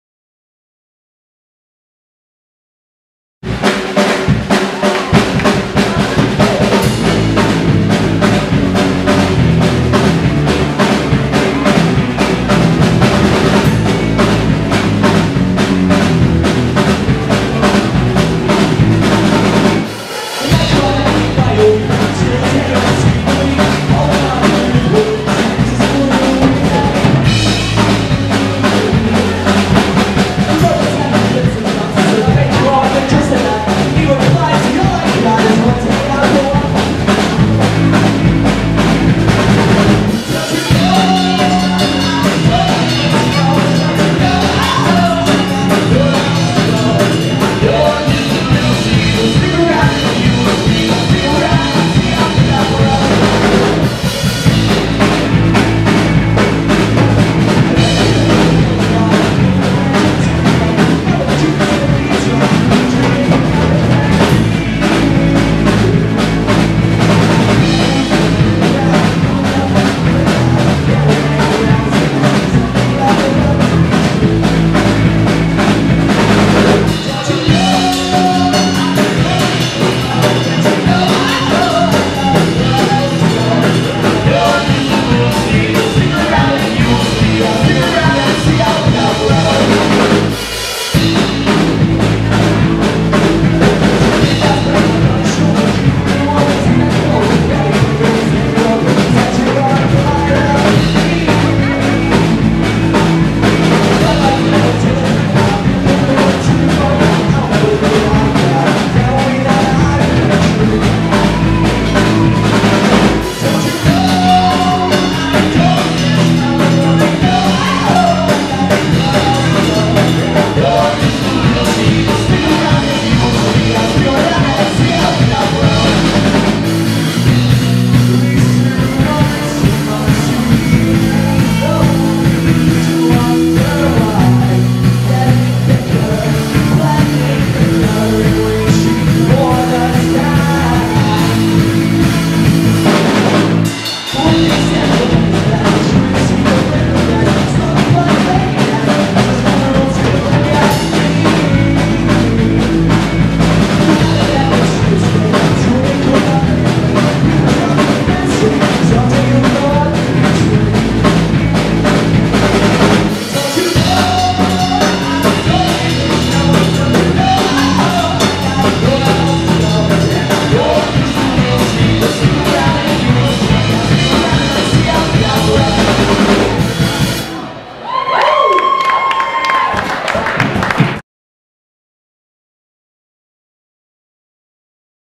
BARRICADES SINGING LIVE AT MCFADDEN'S
BARRICADES Performing live at the MCFADDEN'S in BOSTON